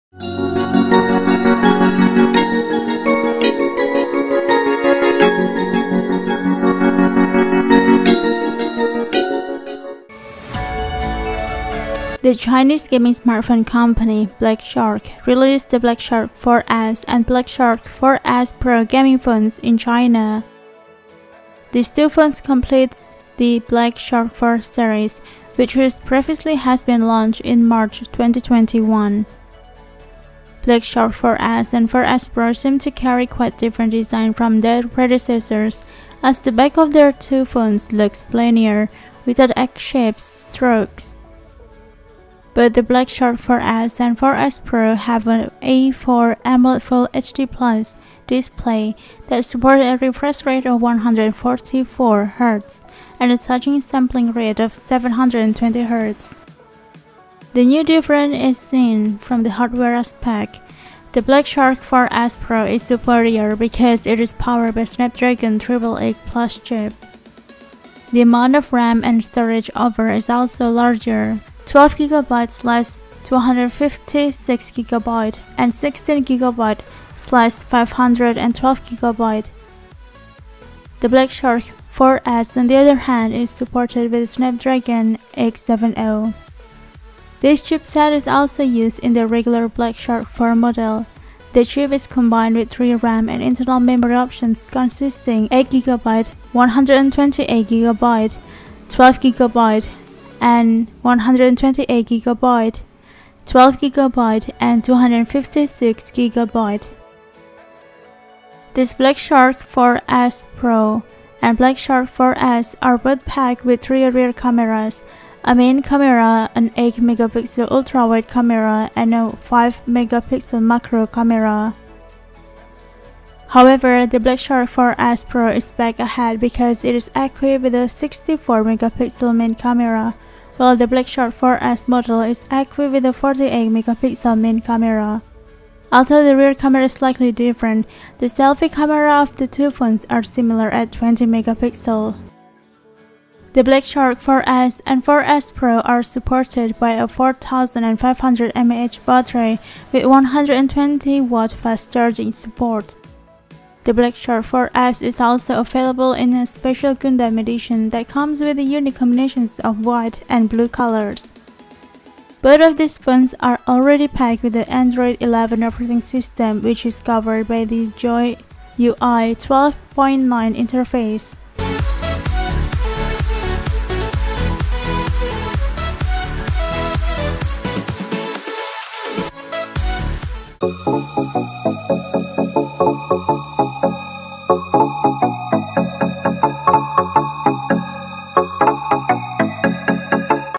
Everything We Know About Black Shark 4S And 4S Pro Gaming Phones
The Chinese gaming smartphone company, Black Shark, released the Black Shark 4S and Black Shark 4S Pro gaming phones in China. These two phones completes the Black Shark 4 Series, which was previously has been launched in March 2021. Black Shark 4S and 4S Pro seem to carry quite a different design from their predecessors as the back of the two phones looks plainer without X-shaped strokes.

Both the Black Shark 4S and 4S Pro have an E4 AMOLED Full HD Plus (1,080 x 2,400 pixels) display that supports a refresh rate of 144 Hz, and a touch sampling rate of 720 Hz. The new difference is seen from the hardware aspect. The Black Shark 4S Pro is superior because it is powered by the Snapdragon 888 Plus chip.

The amount of RAM and storage offered is also larger, 12GB/256GB and 16GB/512GB. The Black Shark 4S on the other hand is supported with a Snapdragon 870. This chipset is also used in the "regular" Black Shark 4 model. The chip is combined with three RAM and internal memory options consisting of 8GB/128GB, 12GB/128GB, 12GB/256GB. These Black Shark 4S Pro and Black Shark 4S are both packed with three rear cameras; a main camera, an 8 MP ultra-wide camera and a 5 MP macro camera.

However, the Black Shark 4S Pro is back ahead because it is equipped with a 64 MP main camera while the Black Shark 4S model is equipped with a 48 MP main camera. Although the rear camera is slightly different, the selfie camera of the two phones are similar at 20 MP.

The Black Shark 4S and 4S Pro are supported by a 4,500 mAh battery with 120 Watt fast charging support. The Black Shark 4S is also available in a special Gundam Edition that come with a unique combination of white and blue colors. Both of these phones are already packed with the Android 11 operating system which is covered by the JoyUI 12.9 interface.